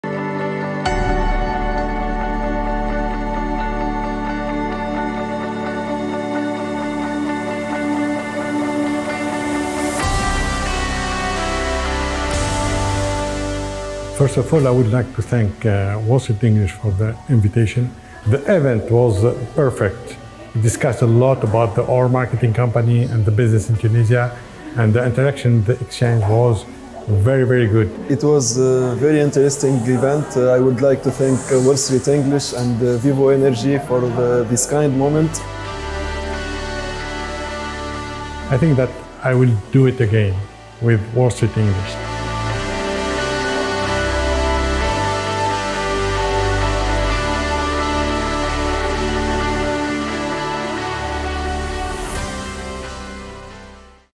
Pratiquez votre anglais avec les événements de Wall Street English Tunisia.
Nous avons eu l'honneur d'inviter notre guest speaker Mr Mohamed Bougriba le PDG de Vivo Energie à notre centre Wall Street English Megrine.
Merci Mr Mohamed Bougriba  d'avoir rendu cet événement spécial
Merci à ceux qui ont rendu cet événement exceptionnel.